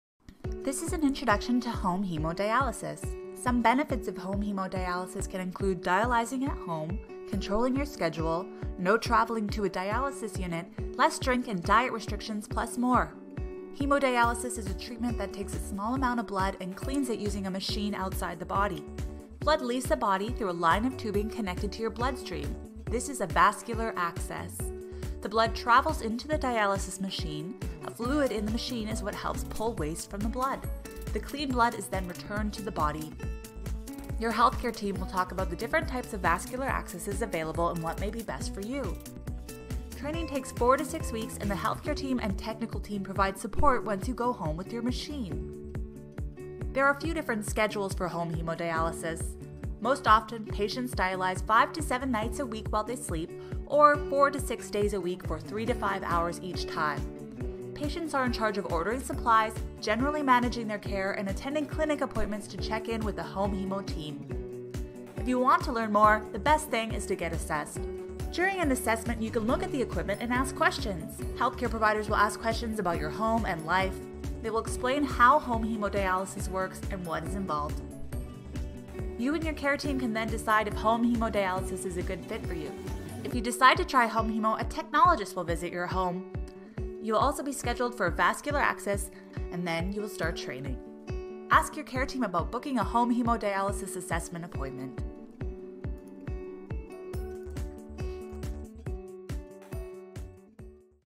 Introducing Home Hemodialysis for Manitoba Renal Patients
This video is a short introduction to home hemodialysis as a treatment option for someone approaching or living with kidney failure. Visit kidneyhealth.ca to learn more.